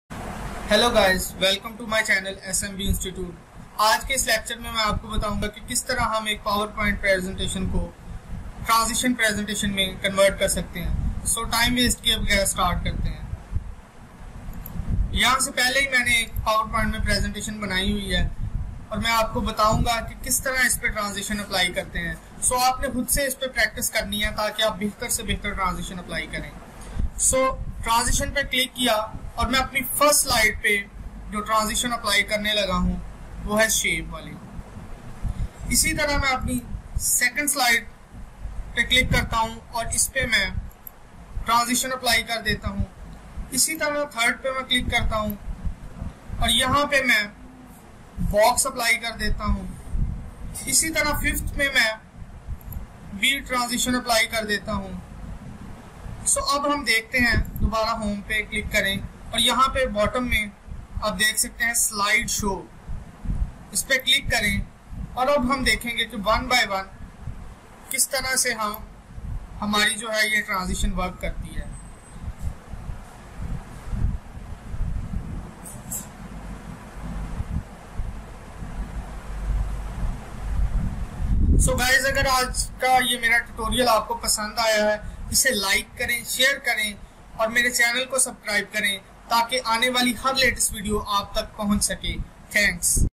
How to Create a Transition Presentation in Urdu/Hindi [PowerPoint Transition]
In this video I will show you how to add transition in PowerPoint.